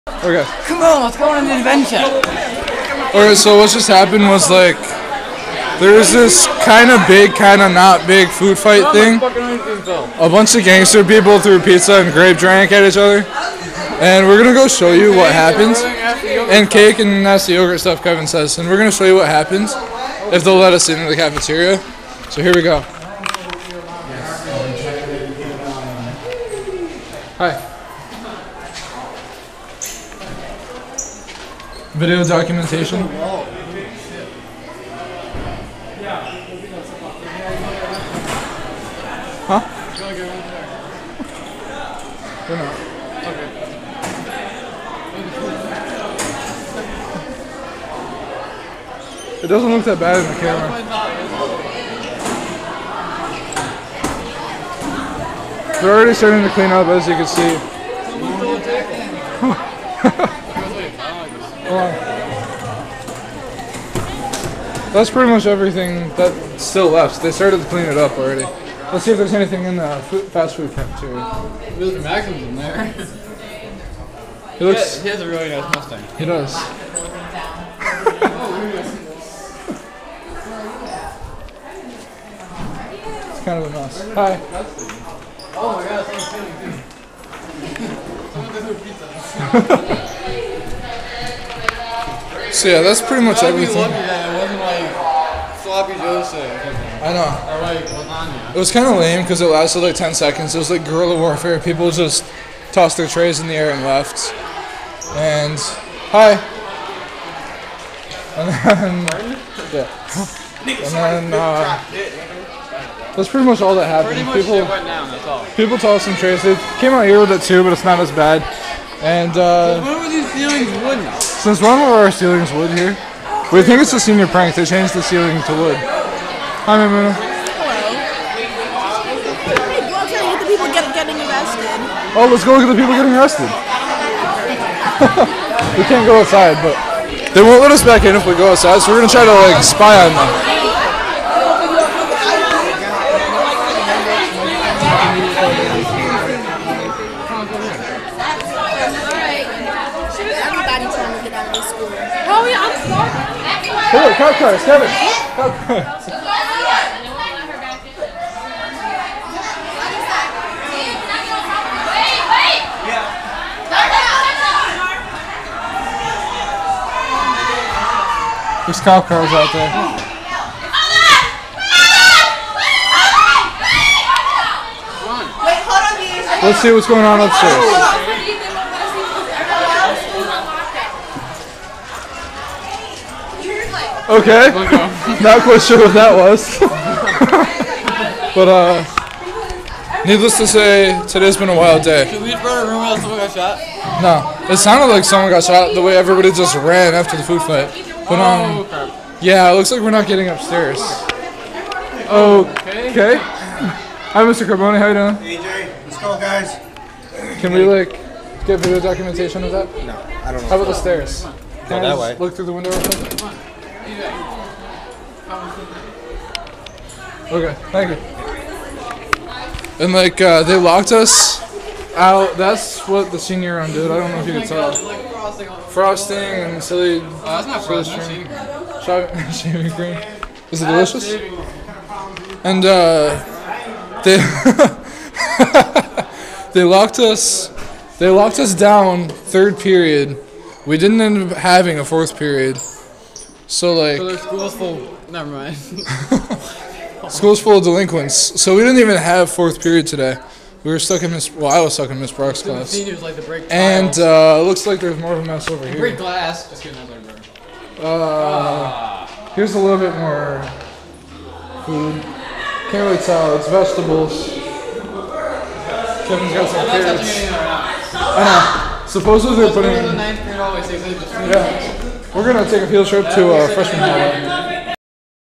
Binghamton High School Food Fight Aftermath 6/10/10
Food fight in school turned into a brawl outside of school, turned into national news and the reason for cops, troopers, SWAT plus more... Unfortunately, we did not get the fight, nor the food fight, on camera. Just the cleanup pretty much.